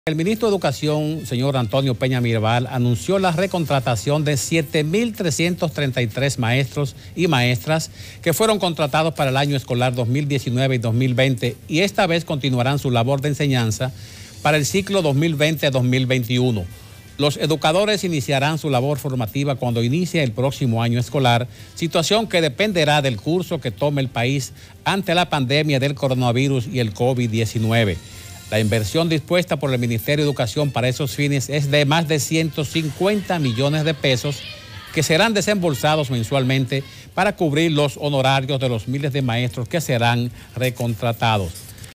Ministerio de Educación recontratará más de 7 mil profesores para nuevo año escolar
El ministro de Educación, Antonio Peña Mirabal, anunció la recontratación de 7 mil 337 maestros y maestras que fueron contratados para el año escolar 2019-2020 y esta vez continuarán su labor de enseñanza para el ciclo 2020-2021.